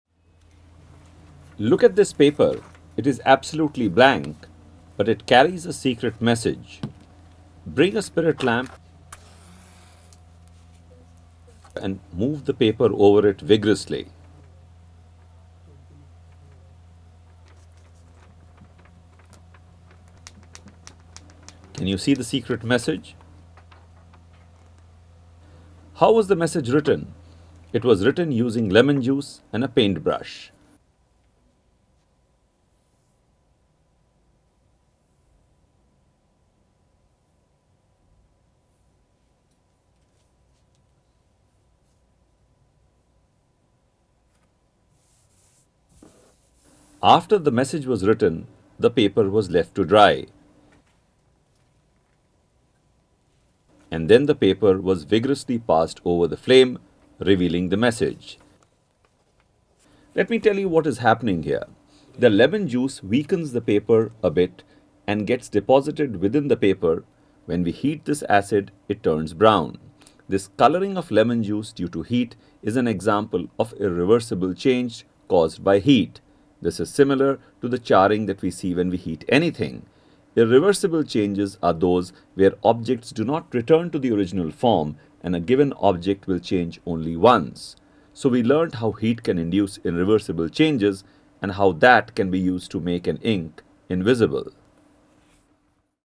Lemon juice turns brown on heating | Irreversible change | Chemistry
In this activity, we see an example of an irreversible change. An irreversible change is a change where an object cannot be taken back to its original state. We vigorously heat a piece of paper on which we have written a secret message using lemon juice. We see that as the paper is heated, the message appears as brown writing. This happens because heat turns the acid present in the lemon juice brown.